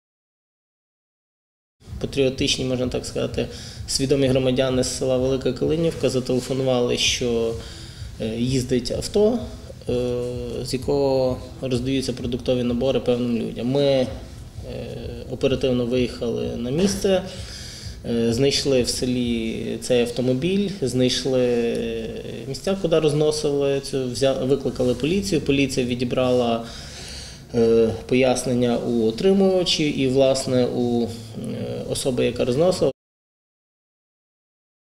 «Опора» зафіксувала спробу підкупу виборців в Олешинській тергромаді
Напередодні виборів у семи об’єднаних тергромадах Хмельниччини представники «Опори» зафіксували факт підкупу виборців. Це сталося  у селі Велика Калинівка Олешинської ОТГ. Тамтешній жительці вручили пакет з продуктами – макаронами – олією, цукром, борошном й крупою та агітували голосувати за певних кандидатів